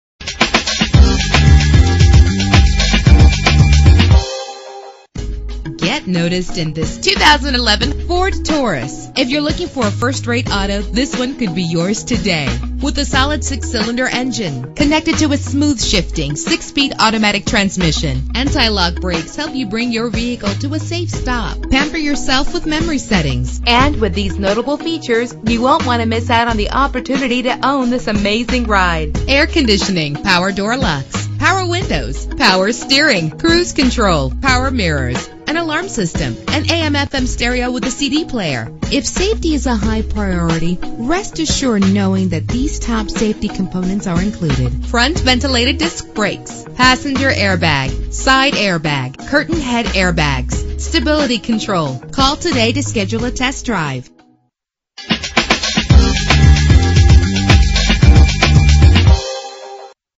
2011 Ford Taurus - Tampa FL
Year: 2011
Make: Ford
Model: Taurus
Trim: Limited
Engine: 3.5 liter 6 cylinder 24 valve
Transmission: 6-Speed Automatic
Color: Sterling Gray
Mileage: 7
Address:
7201 West Linebaugh Avenue